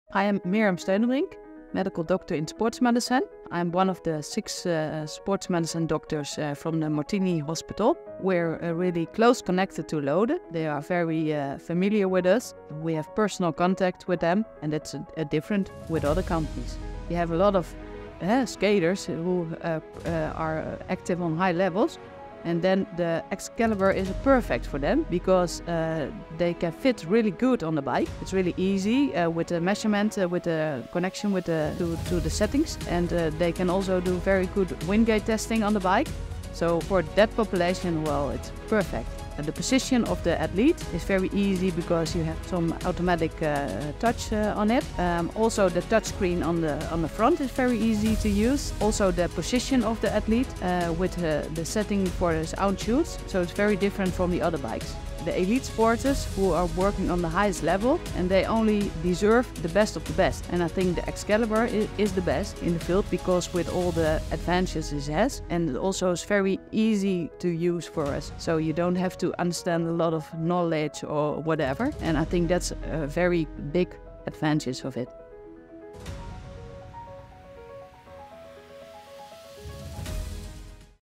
Testimonial Mirjam Steunebrink Excalibur Japanese subtitles
In this video, Mirjam Steunebrink, a medical doctor in sports medicine at Martini Hospital, talks about the Excalibur from Lode.